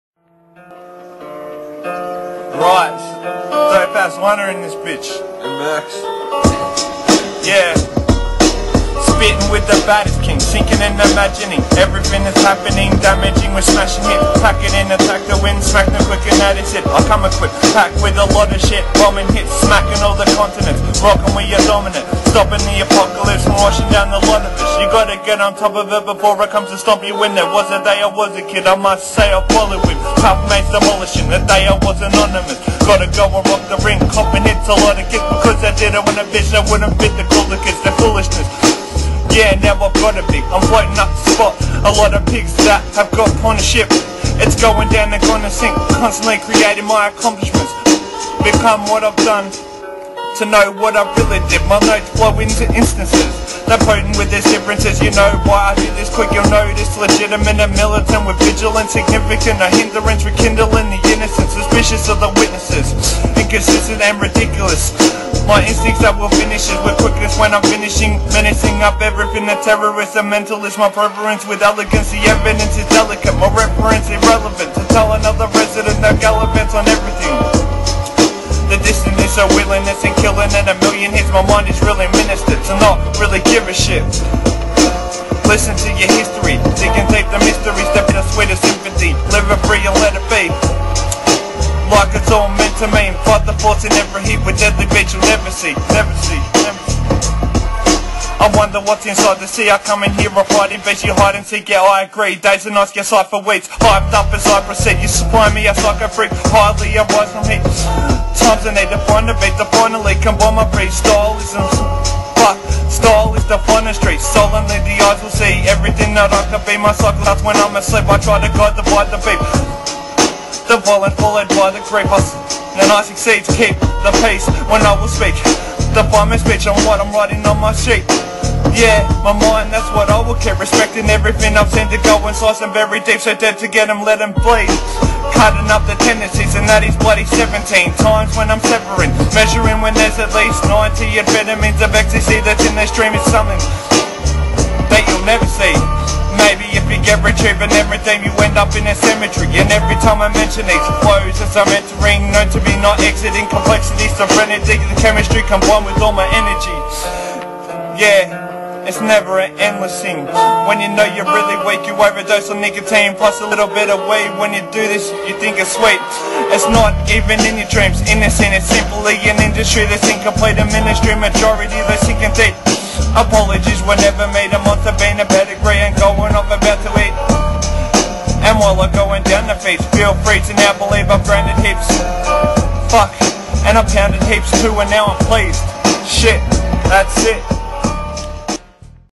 Dopaz demo
my mate Dopaz chuckin a rap :)